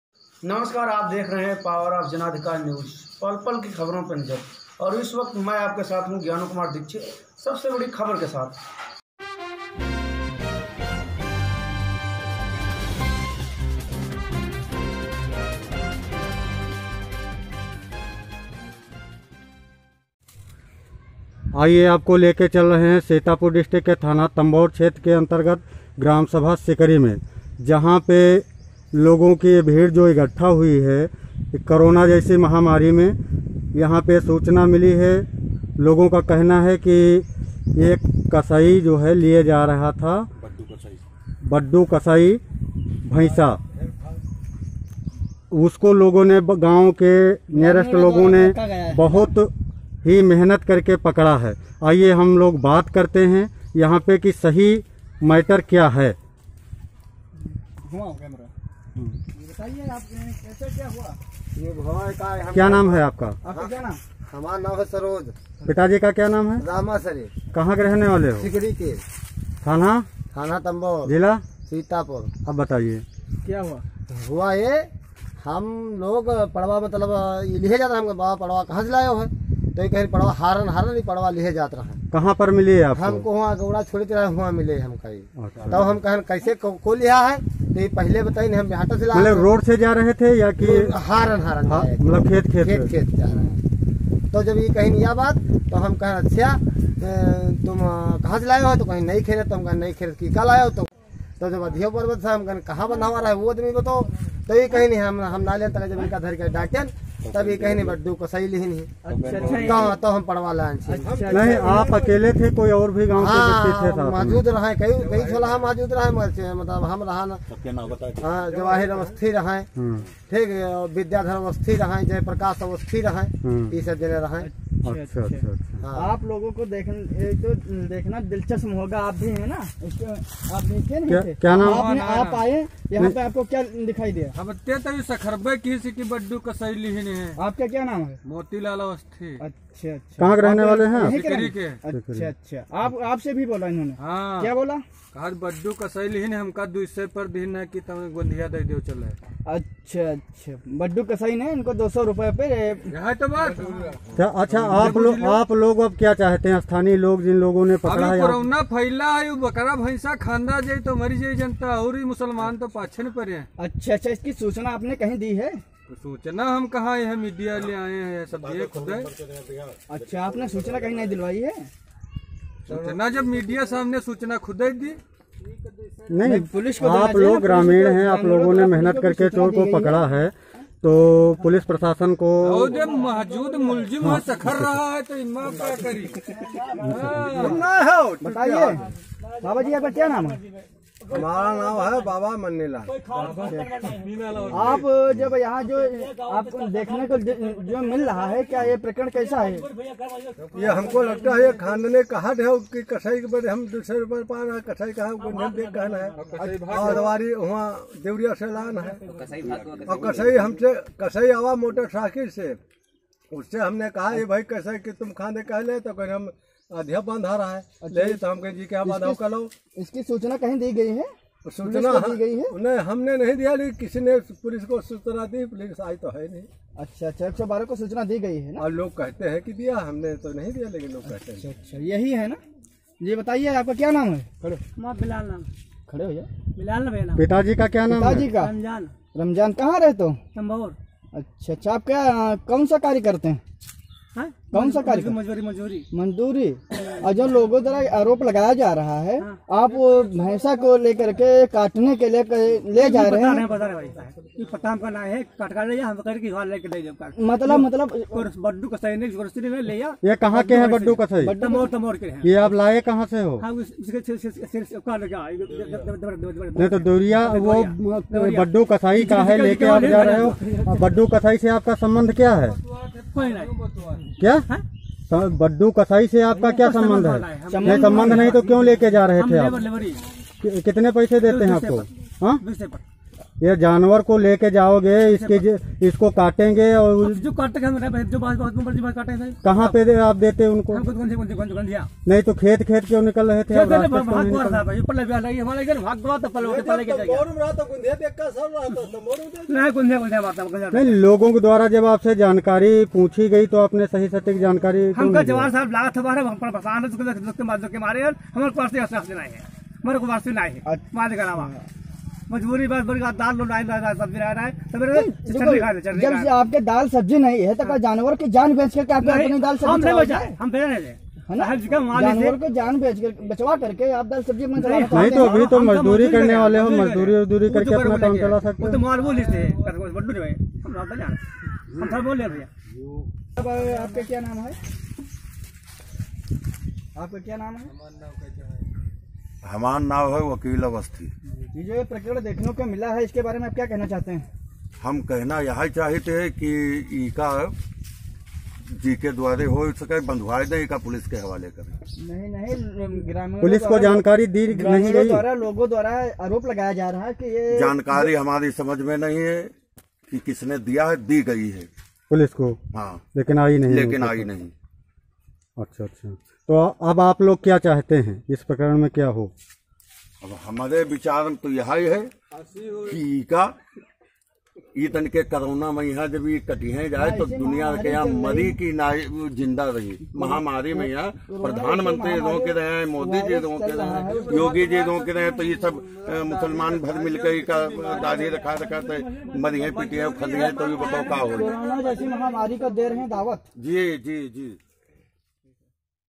*सीतापुर करोना वायरस सर पर नाच रहा है और इनको भैंसा खाना हैं* काटने हेतु तंबौर के बड्डू कसाई अपने
सीतापुर भैंसा काटने हेतु तंबौर के बड्डू कसाई अपने नौकर के साथ भैंसा लिए जा रहे थे कि तभी गांव के लोग खेतों में पर मौजूद थे सिकरी गांव के लोगों को देखकर नौकर बड्डू कसाई भागे तभी गांव के कुछ व्यक्तियों ने दौड़कर भैंसा वा नौकर को पकड़ लिया बड्डू कसाई भागने में कामयाब रहे मीडिया को यह जानकारी सरोज पुत्र रामासरे ने दिया मौके पर ग्रामीणों फोन तो घंटो मिलाने पर भी 112 घंटों मिलाया लेकिन लेकिन नहीं लगा फोन इस पर शुभ करन मौर्य न्यूज़ पोर्टल के पत्रकार मुलीला ने तंबौर पुलिस से वार्ता किया पुलिस आने से इनकार किया और खा भैसा काटने की छूट है तब बड्डू कसाई ने वकील अवस्थी के निज निवास पर पहुंचे अवस्थी से हुई वार्ता वकील अवस्थी ने छुड़वाया भैंसा आखिर नही बचा सके काटने के लिएले गया शर्म की बात है समस्त ग्रामवासी एक जानवर की जान को बचाने में रहे नाकाम मौके पर  तंबौर पुलिस ने किया आने से इनकार
हरिओम सुक्ला की रिपोर्ट बेहटा सीतापुर